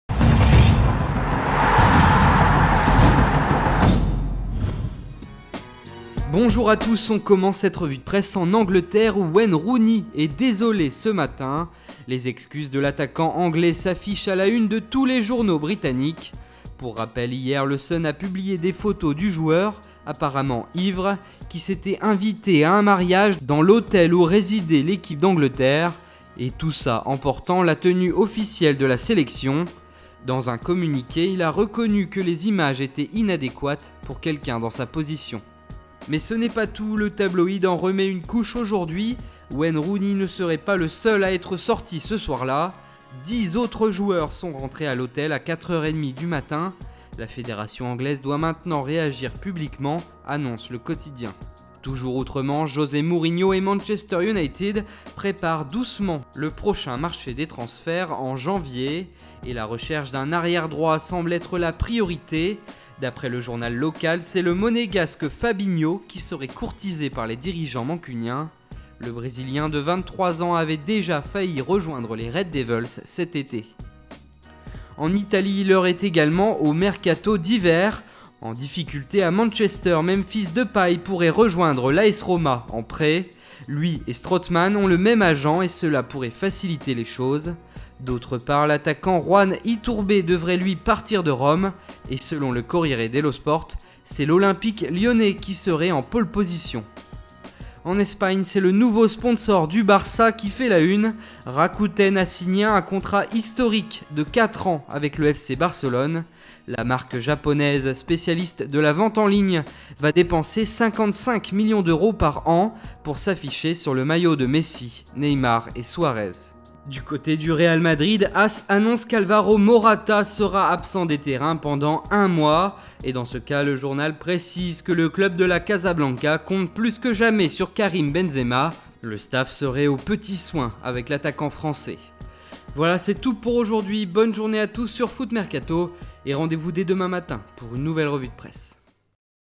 Le nouveau sponsor du Barça qui va rapporter gros | Revue de presse
Mourinho veut un Monégasque en janvier, Lyon se place pour un attaquant en Italie ou encore les dérapages qui continuent en Angleterre, retrouvez dans votre revue de presse Foot Mercato les dernières informations de la presse sportive européenne.